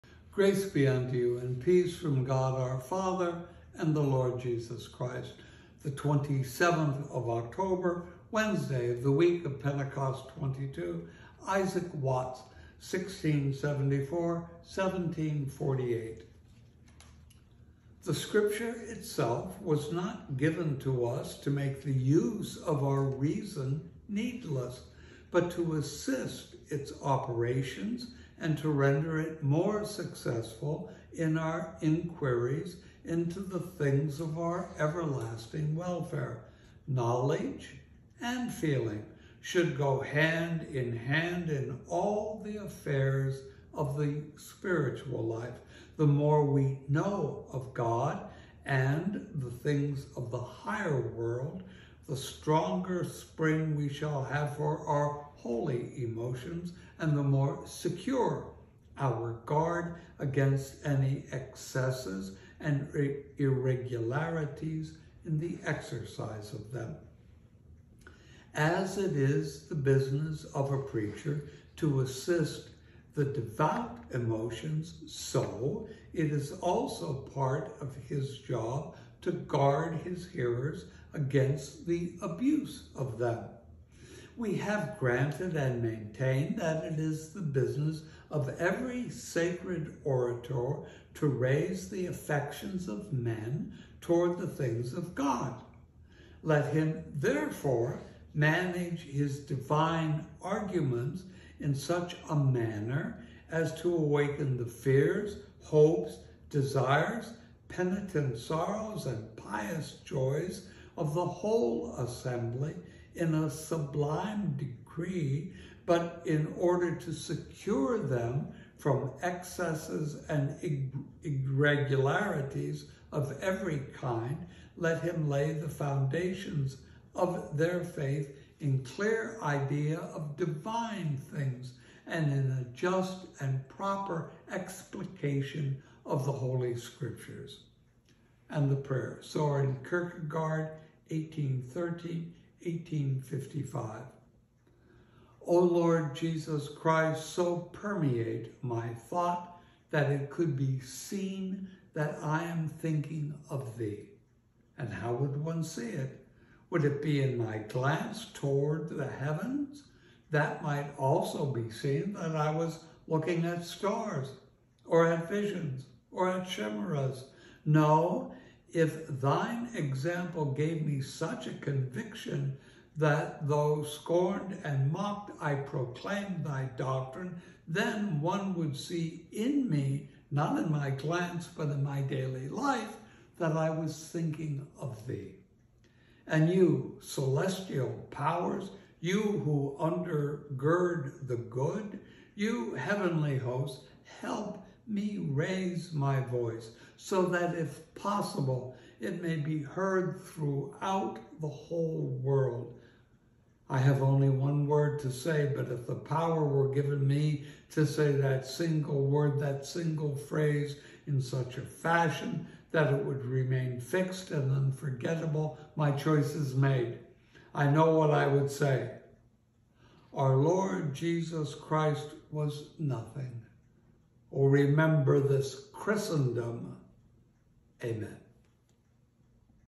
27 October 2021 Wednesday of the week of Pentecost XXII
A reflection on ’Emotions in spiritual life’ Isaac Watts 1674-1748. Prayer: Soren Kierkegaard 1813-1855. For All The Saints Volume II